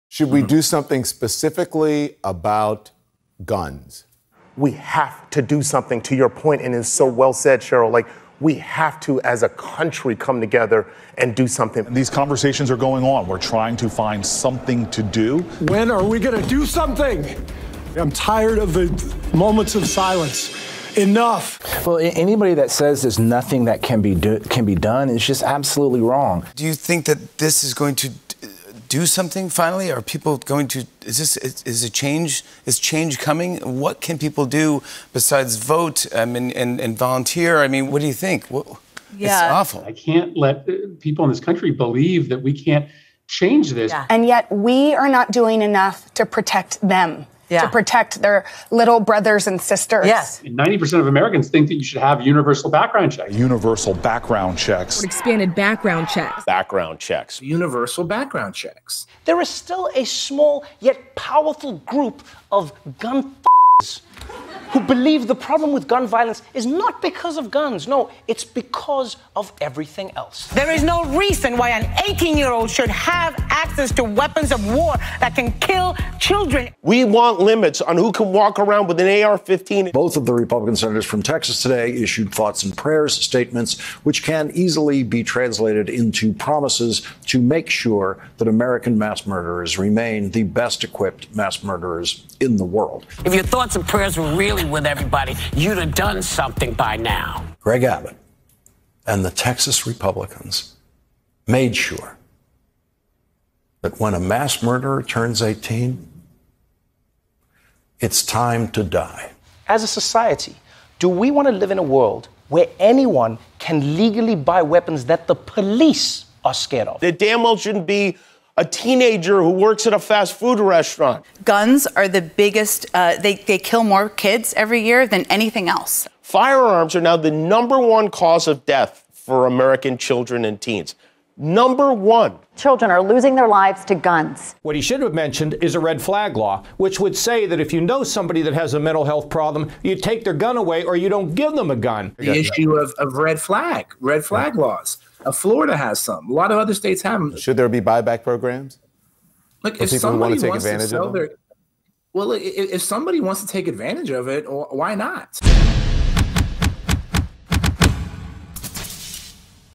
"Weapons of War!": Celebrities, Media, Politicians Stoke Fear, Push Gun Control
Many members of the media and politicians came out against Americans' 2nd Amendment rights after the shooting in Uvalde, Texas.